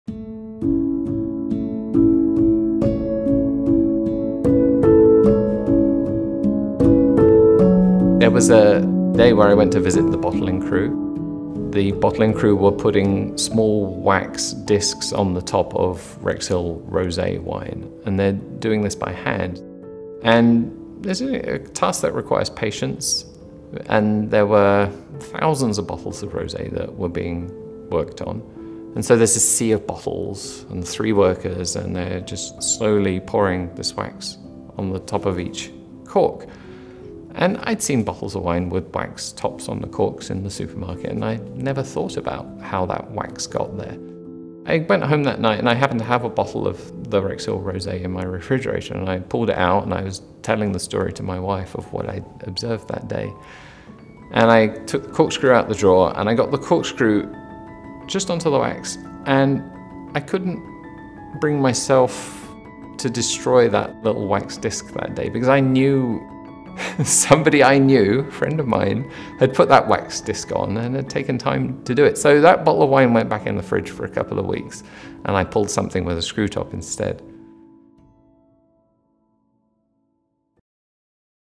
A to Z Wineworks: Wax
We distill your big ideas into short, authentic films.

If you’ve ever wondered how the wax gets applied to the wine cork look no further.  One Hundred Seconds teamed up with A to Z Winework to highlight one of the many aspects that makes their wine truly unique.